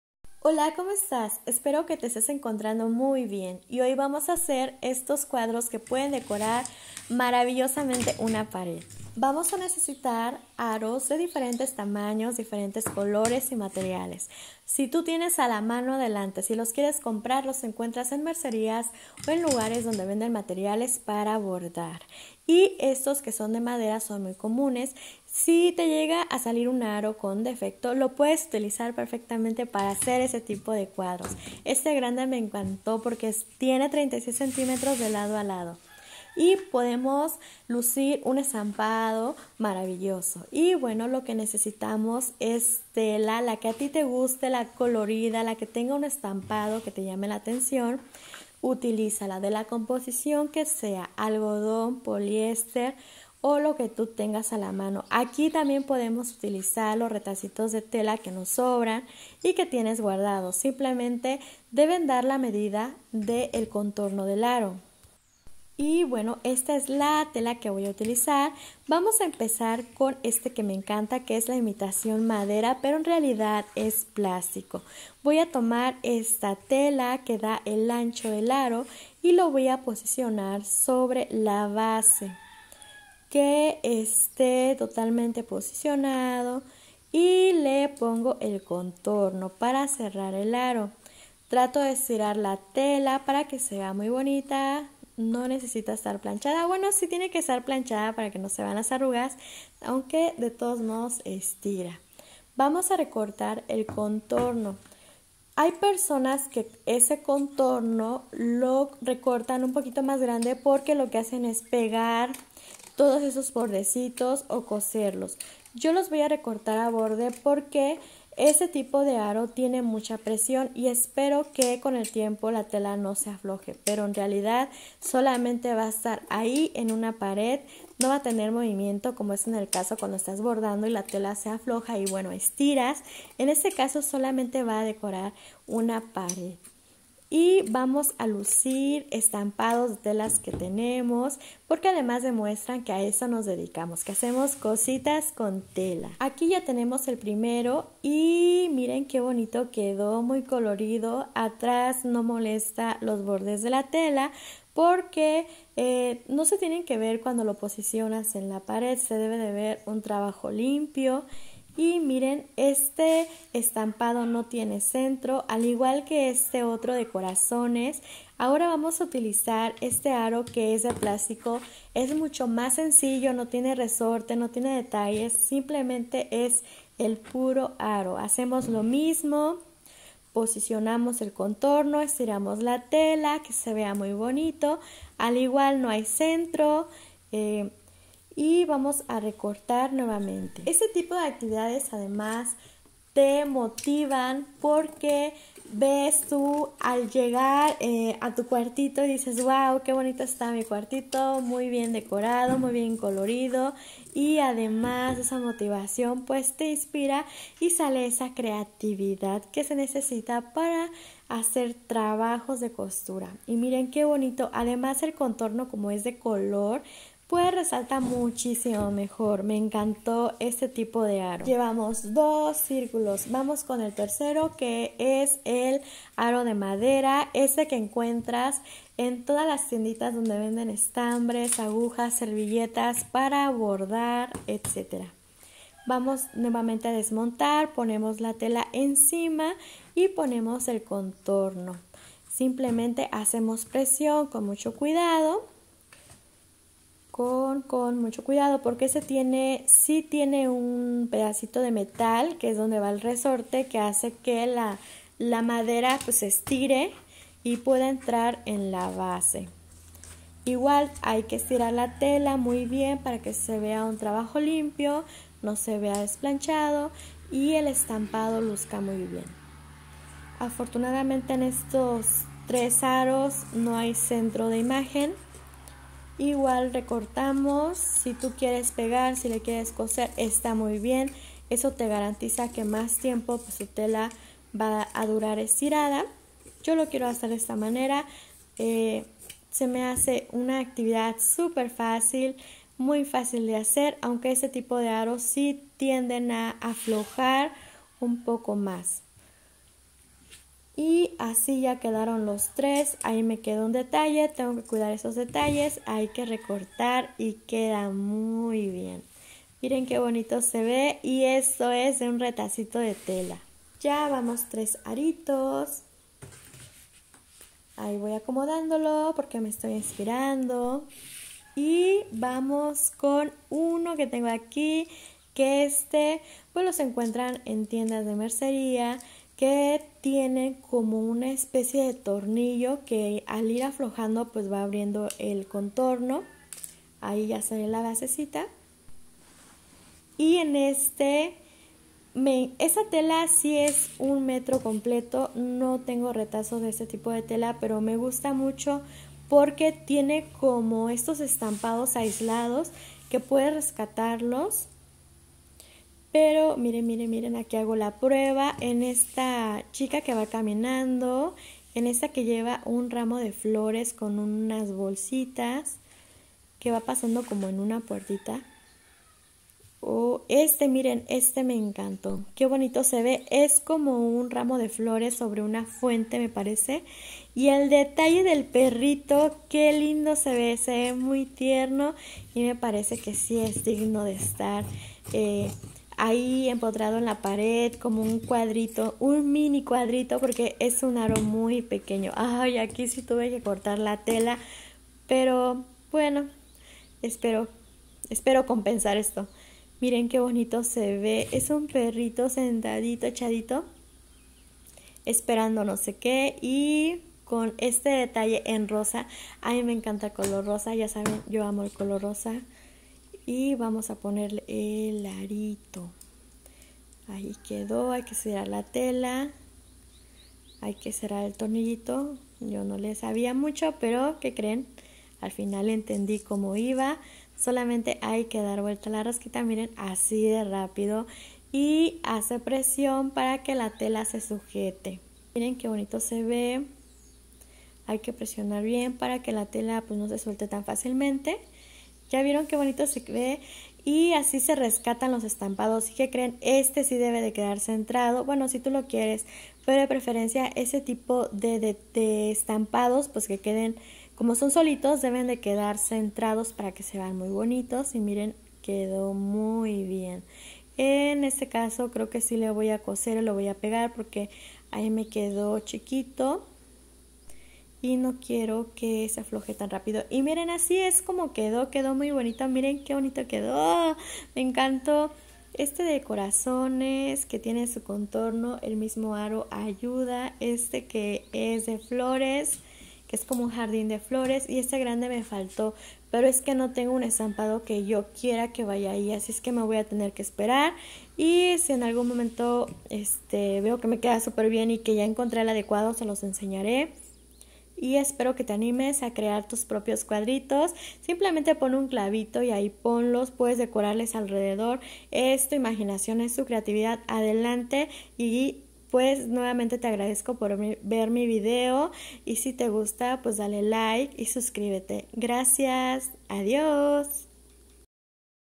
¿Como hacer aros decorativos para cuarto de costura? 🖼️
Elementos para decorar un cuarto de costura super fácil y muy creativo.